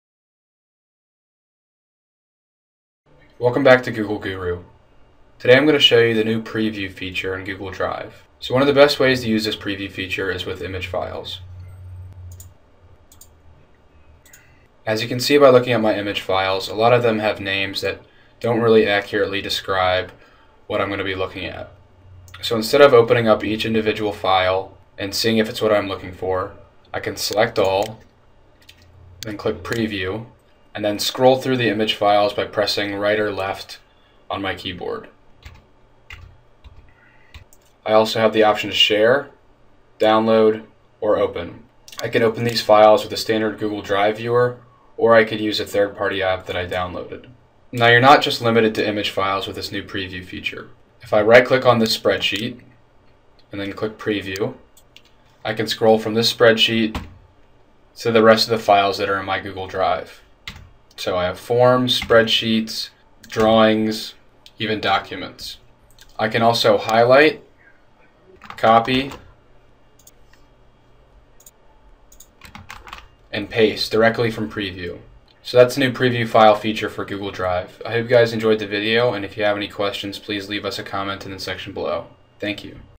Preview files in Google Drive
The new Preview file feature for Google Drive makes previewing files easier than ever before. You can now preview more than 30 types of files and easily switch between them until you find the one you want. You can even copy text while previewing a file! Learn more in this Google Drive Update video.